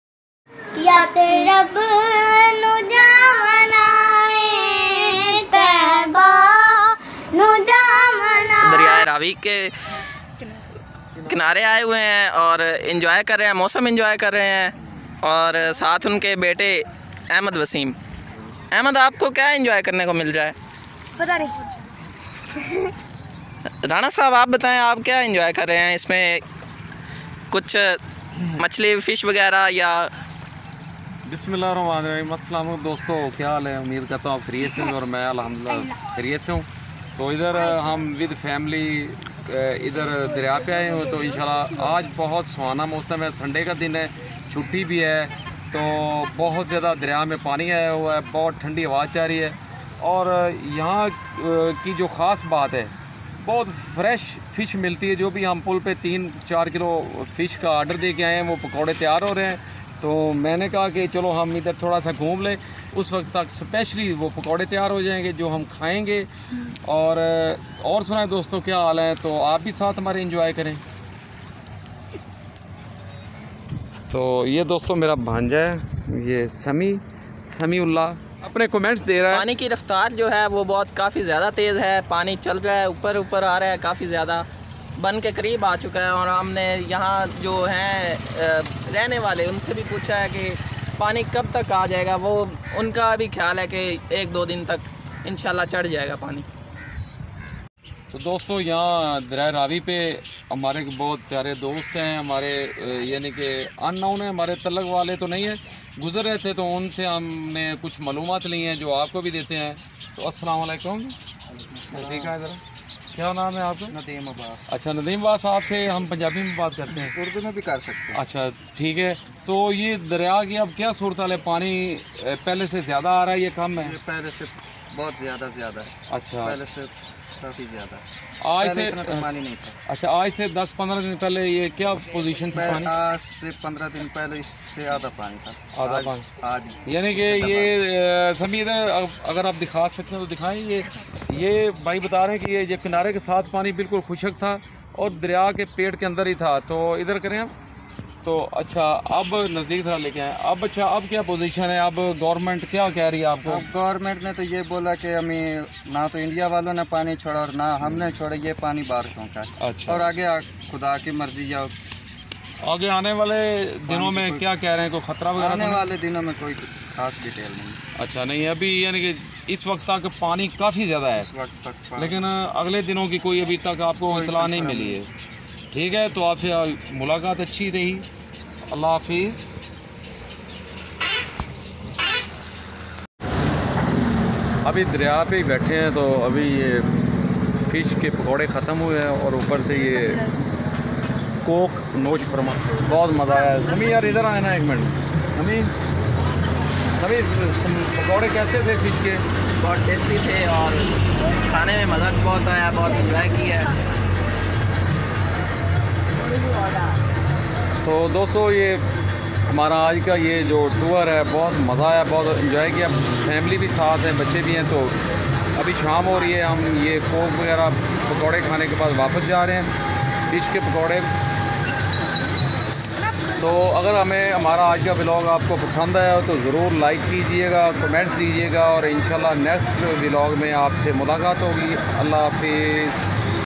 Family Picnic at Ravi River | Beautiful Day Out with Loved Ones ❤️
Join us for a serene and fun-filled day at the Ravi River! 🌊 We spent quality time with the family, enjoying the peaceful environment and natural beauty. From peaceful boat rides to scenic views, this outing was all about making memories and bonding with loved ones. Watch our journey unfold as we explore the serene beauty of Ravi River together! 😊"